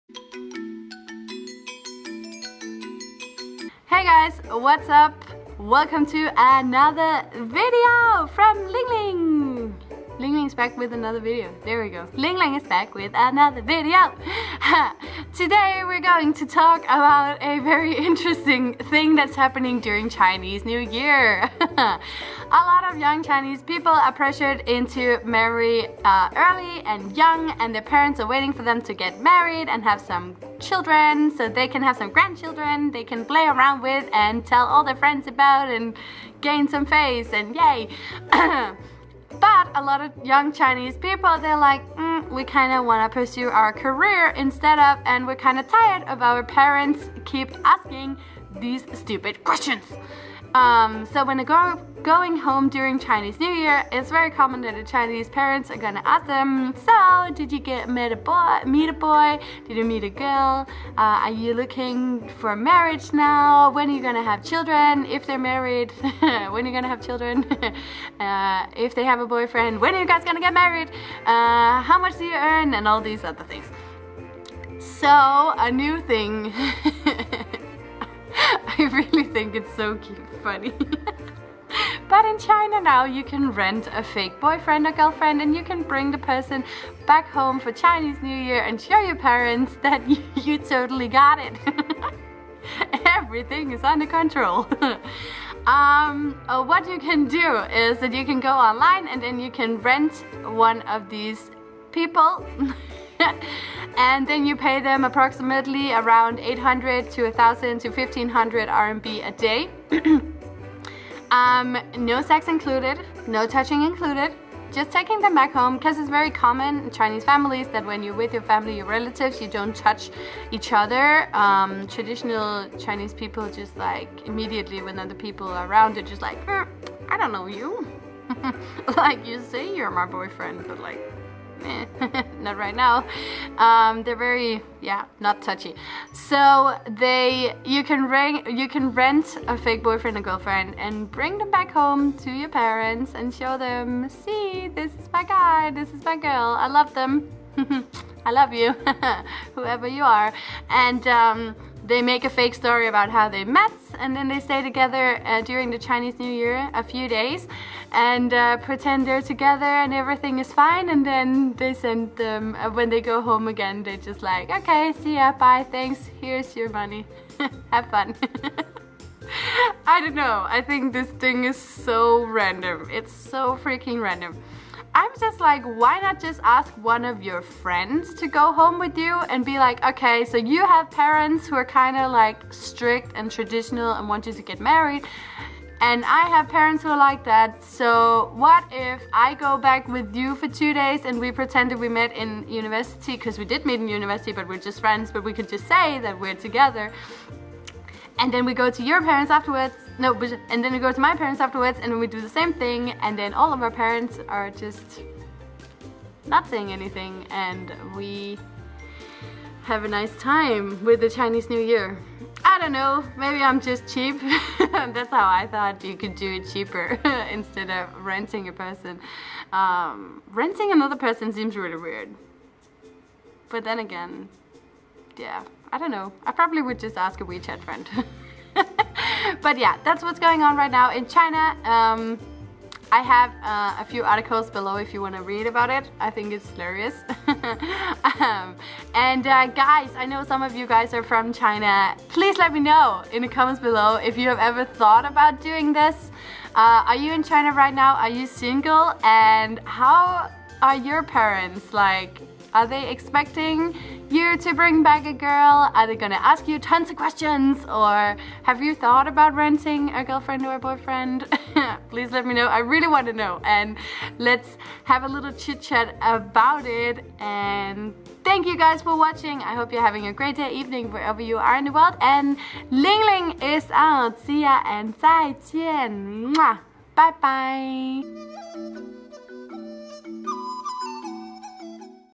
Rent a Boyfriend for Chinese New Year?! // 春节回老家租一个男朋友
Hey guys!

I read about this and thought I would share it with you guys. Let me know if you have ever dealt with this or what do you think?

Lingling